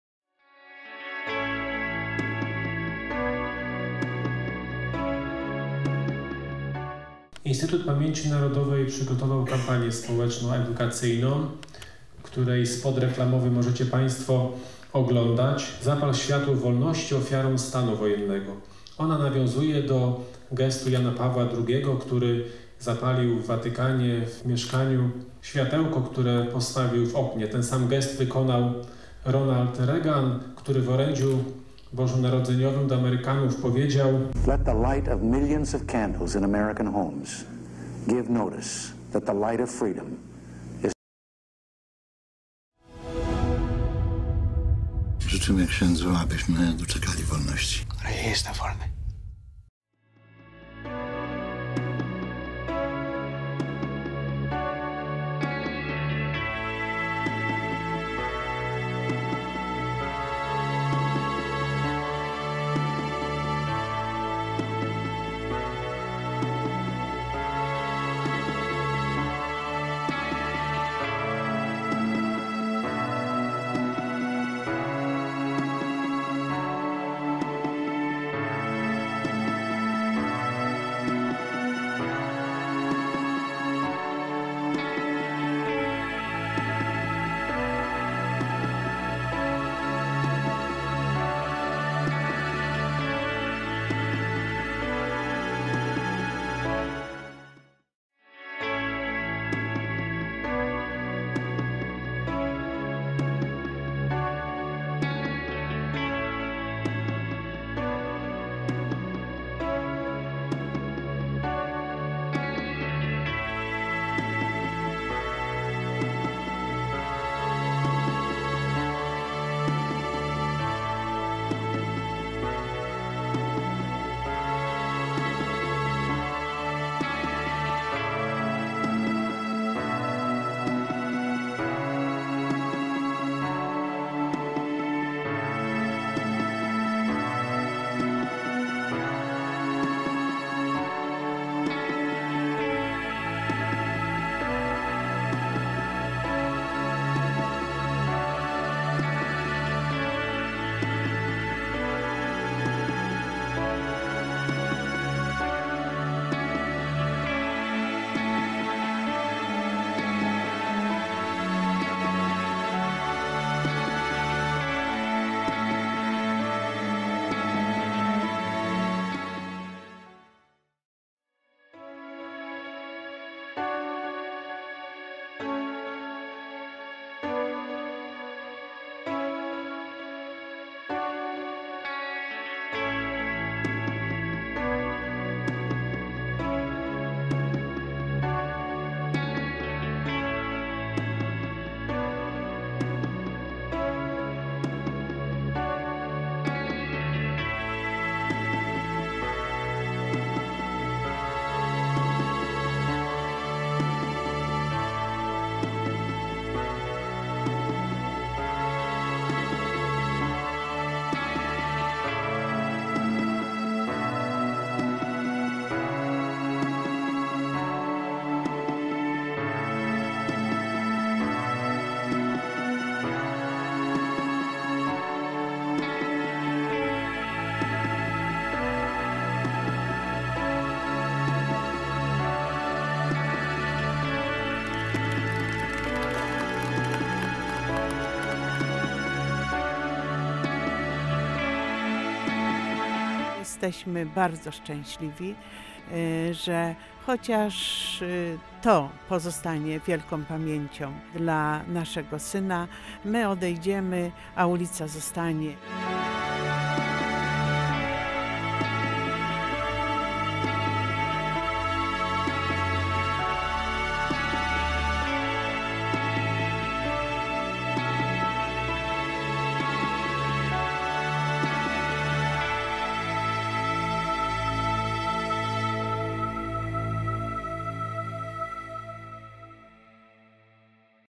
IPN TV Poznań - Obchody 33. rocznicy wprowadzenia stanu wojennego
Poznańskie obchody 33. rocznicy wprowadzenia w Polsce stanu wojennego połączone z akcją IPN "Ofiarom stanu wojennego. Zapal Światło Wolności".

Realizacja i montaż: Bartosz Duleba.